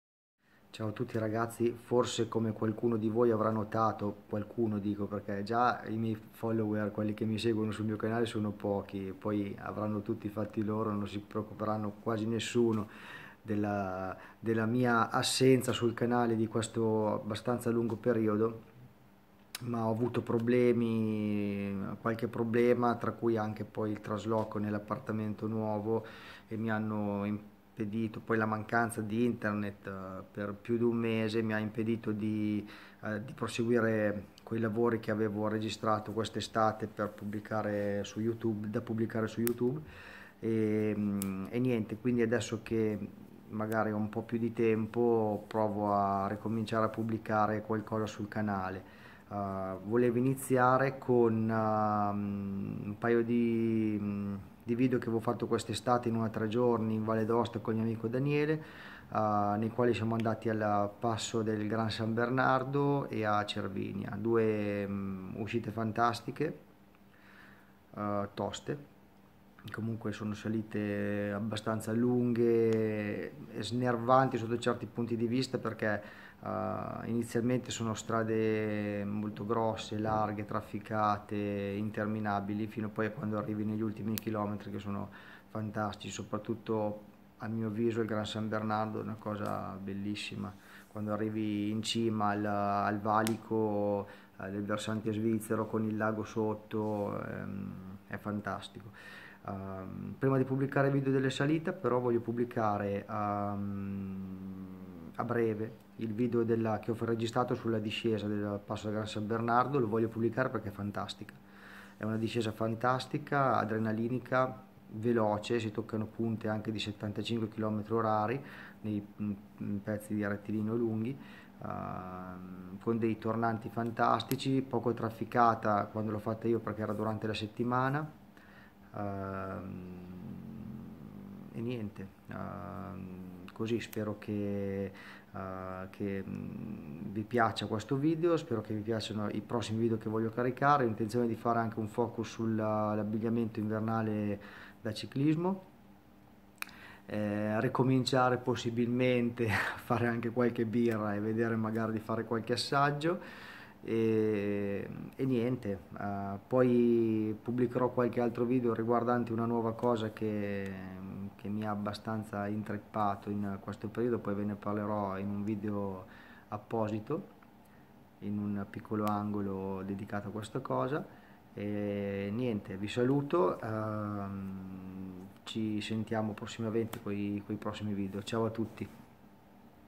CIAO A TUTTI...... facciamo quattro chiacchiere insieme
Ciaoo a tutti ragazzi, dopo una prolungata assenza dal canale senza caricare nulla di significativo, per svariati motivi che non sto ad elencare ma tra i quali c'è anche stato il trasloco nel nuovo appartamento con annessi e connessi, non da meno la mancanza della linea internet per un bel po' di tempo.
Controllando sulla Sd Card della GoPro ho trovato i video registrati con Daniele in una uscita in Valle d'Aosta, che cercherò di caricare nel piu' breve tempo possibile; niente di trascendentale ma c sono 2 ascese importanti come Il Passo del Gran San Bernardo e Cervinia,e le voglio condividere, ed in piu' carichero' in primis la discesa del Gran San bernardo. che e' a dir poco Esaltante!!!
Per ora vi saluto. Ciao a tutti e Guardate e commentate i video.
Giovanni